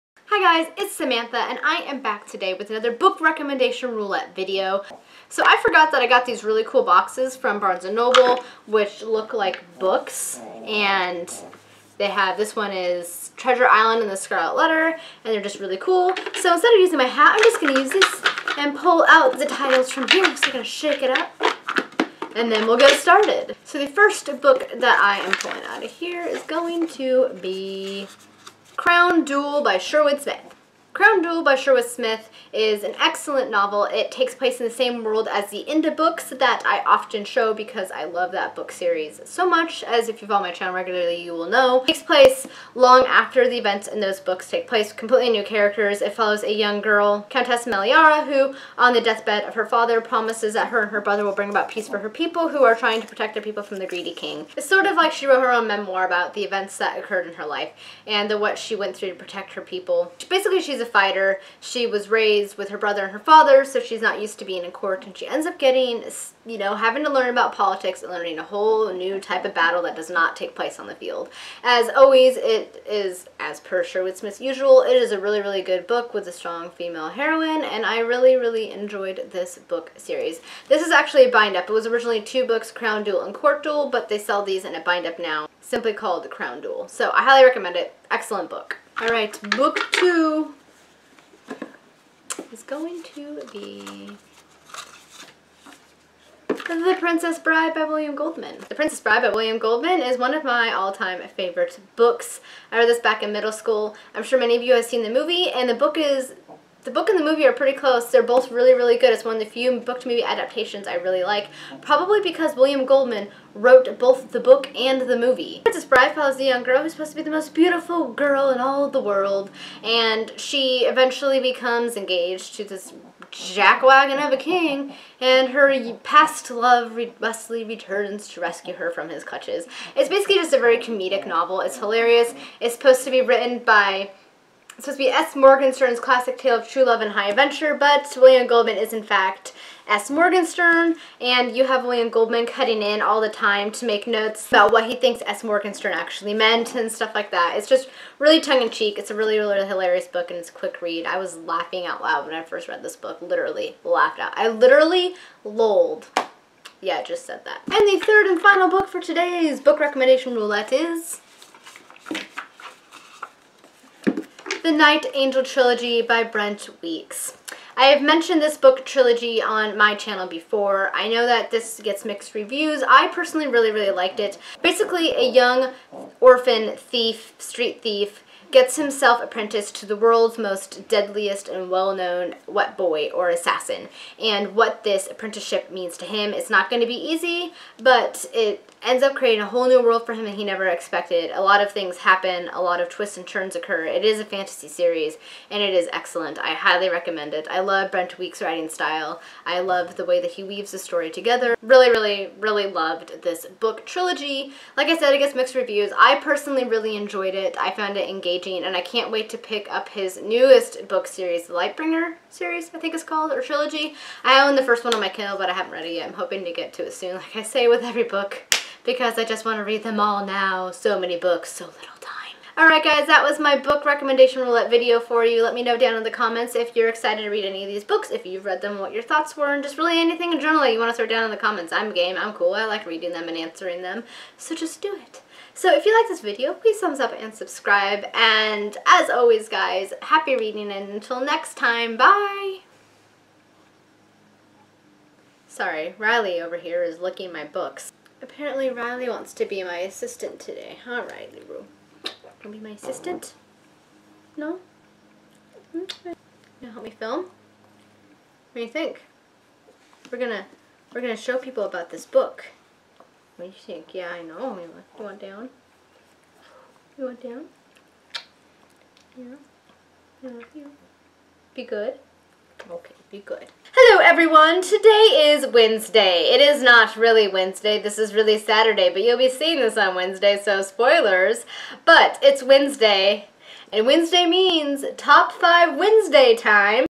Book Recommendation Roulette | #4
Back again with another book recommendation roulette! Let me know if you've read these and what you thought!